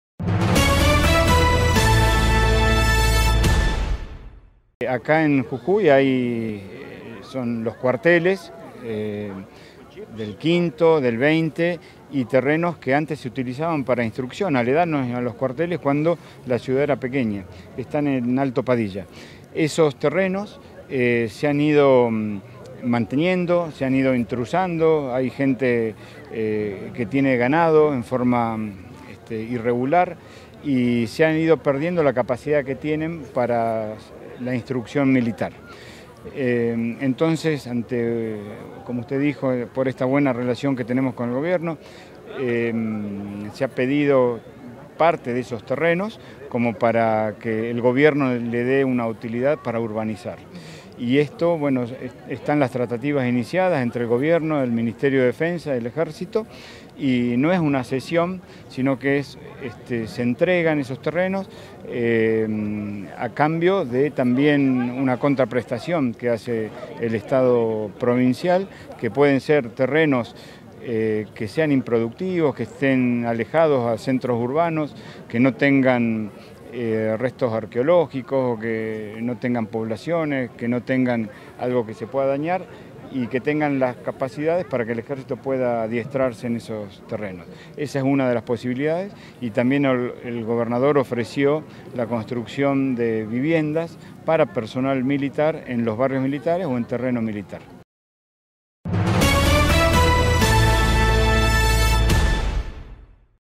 Alto Padilla, ¿el nuevo Alto Comedero?
Los terrenos que están en la zona de Alto Padilla, custodiados por el Ejército, tendrían otro destino al analizarse la posibilidad de que sean cedidos a la provincia para la construcción de unidades habitacionales, de acuerdo al proyecto que todavía está en estudio por parte del gobierno de la provincia y el Ministerio de Defensa de la Nación, ya que esos terrenos son improductivos para el ejercicio militar, aunque se aclaró que no se trata de una cesión, sino de una contraprestación.